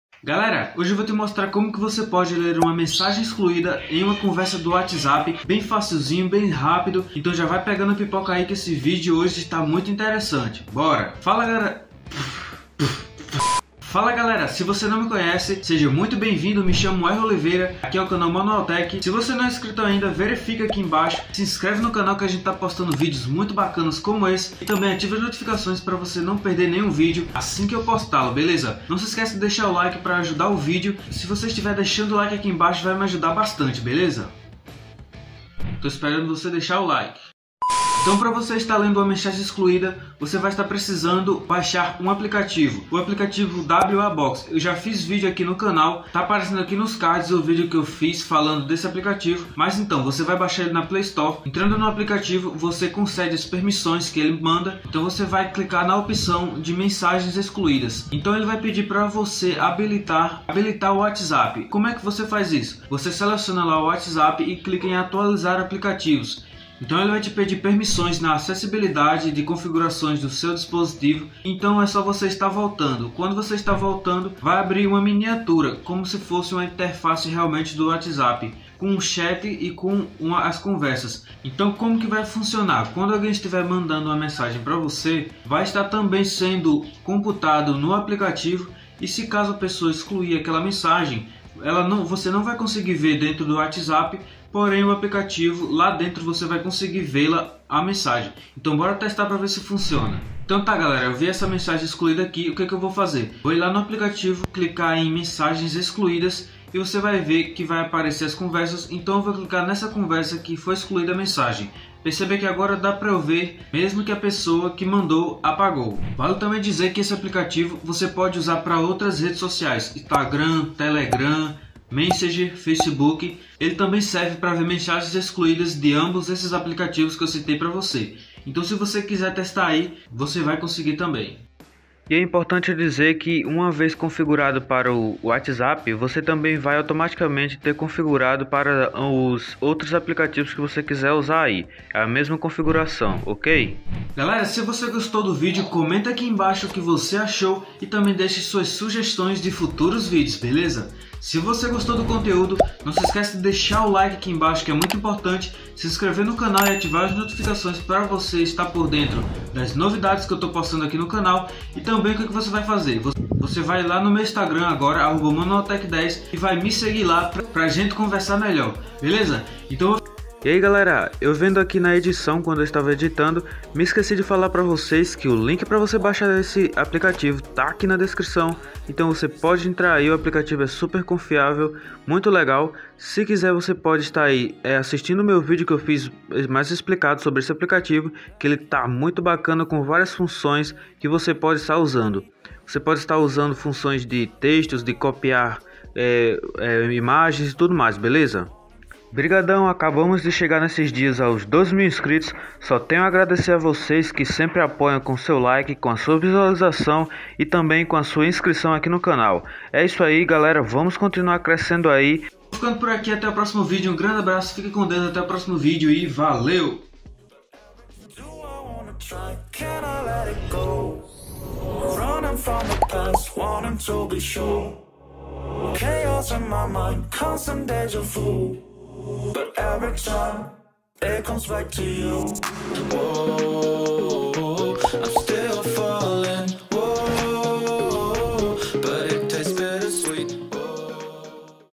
🚨 [INÉDITO] Como Ver Mensagens EXCLUÍDAS no WhatsApp Atualizado 2021!!
Como ler e ver mensagens excluídas/apagadas no WhatsApp. As vezes nós ficamos curiosos após recebermos uma mensagem e em seguida o emissor apagá-la, então nesse vídeo eu te mostro de forma fácil como ver uma mensagem que alguém excluiu no WhatsApp.